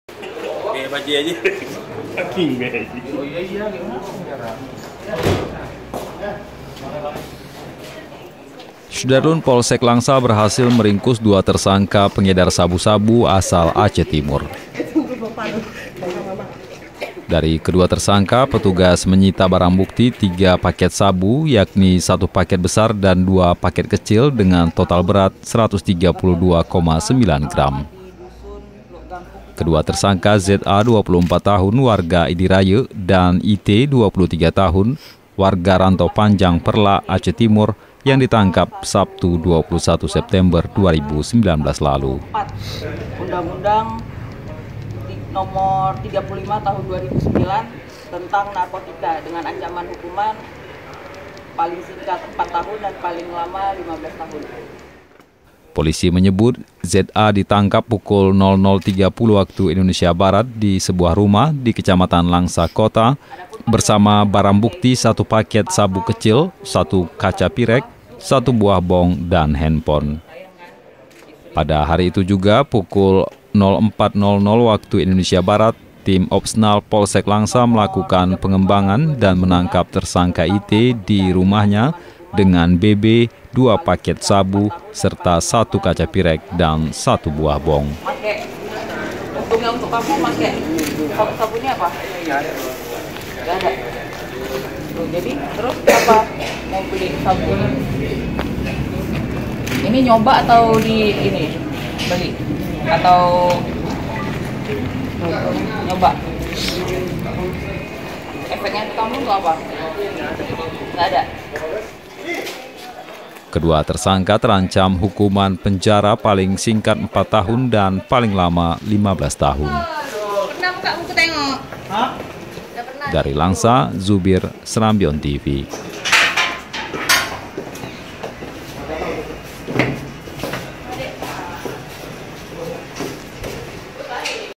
Dua Tersangka Pengedar Sabu Asal Aceh Timur Ditangkap di Langsa
SERAMBITV.COM, LANGSA - Polsek Langsa berhasil meringkus dua tersangka pengedar sabu-sabu asal Aceh Timur.

Dari kedua tersangka petugas menyita barang bukti 3 paket sabu, yakni 1 paket besar dan 2 paket kecil dengan total seberat 132,9 gram.

Kedua tersangka ZA (24) warga Idi Rayeuk, dan IT (23) warga Rantau Panjang Peureulak, Aceh Timur yang ditangkap Sabtu (21/9/2019) lalu.

Polisi menyebut, ZA ditangkap pukul 00.30 WIB di sebuah rumah, di Kecamatan Langsa Kota, bersama BB 1 paket sabu kecil, 1 kaca pyrek, 1 buah bong, dan handphone.

Pada hari itu juga pukul 04.00 WIB Tim Opsnal Polsek Langsa melakukan pengembangan dan menangkap tersangka IT di rumahnya, dengan BB 2 paket sabu, serta 1 kaca pyrek, dan 1 buah bong.

Kedua tersangka terancam hukuman penjara paling singkat 4 tahun dan paling lama 15 tahun.